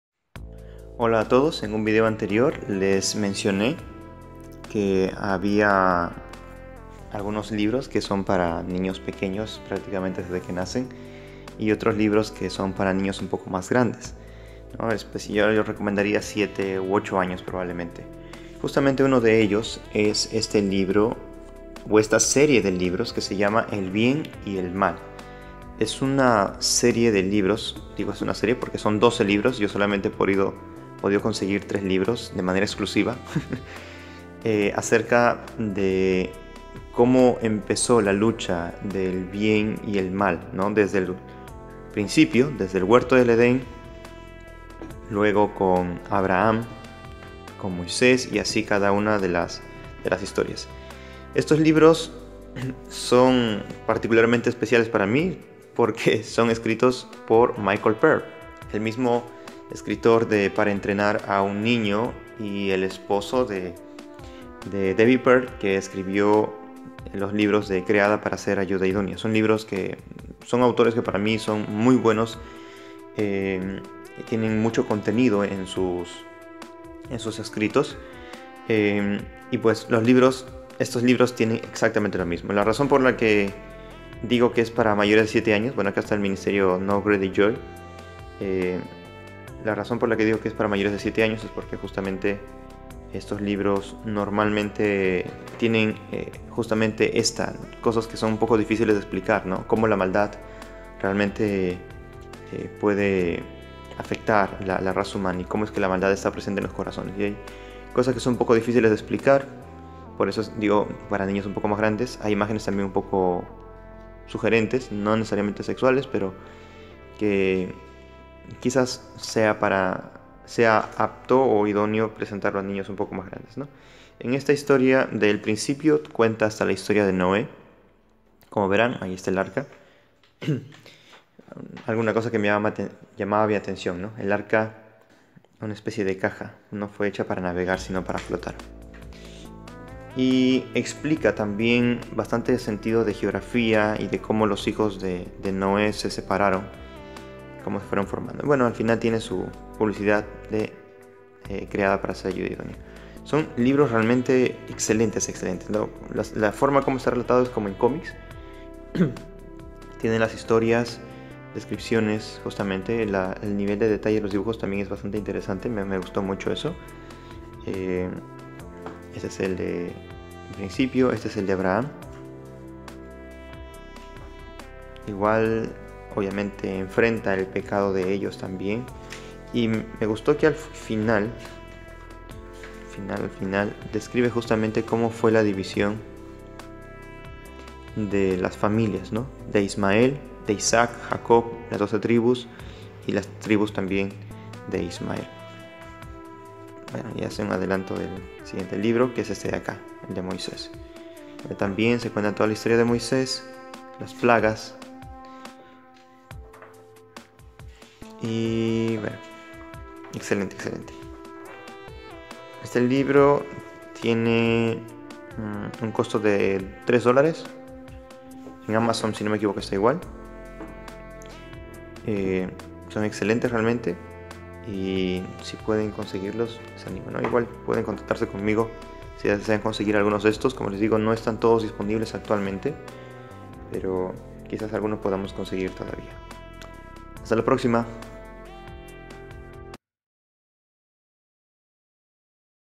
EL BIEN Y EL MAL (La Biblia en Comic para Niños) - Michael Pearl | BookReview por Rubén RuYs
¡La última novela gráfica de superhéroes!
Dios no escogió presentarse ante la humanidad mediante principios, conceptos o doctrina, sino a través de historias de profecía, guerra, misericordia, juicio, milagros, muerte, vida y perdón.
Este sorprendente libro te lleva a través de la Biblia relatando los eventos claves en un formato fácil de captar.

Presenta el plan redentor de Dios narrado cronológicamente desde el Génesis hasta el Apocalipsis.

El bien y el mal aclamado por la crítica (Independent Publishers 2009 Medalla de bronce en la categoría Novela gráfica / Drama y Finalista del premio Forward Book 2009) es verdaderamente la novela gráfica de “superhéroe definitivo” y está teniendo un gran impacto en todo el mundo. Escrito por el autor Michael Pearl (Por Divine Design, Holy Sex, Eight Kingdoms) e ilustrado por el ex artista de Marvel Comics Danny Bulanadi (Fantastic Four, Incredible Hulk, Micronauts) Good & Evil es una presentación bíblica verdaderamente única. Ahora se está traduciendo y distribuyendo en docenas de idiomas, tanto en forma impresa como en línea, y hay docenas más de traducciones en curso.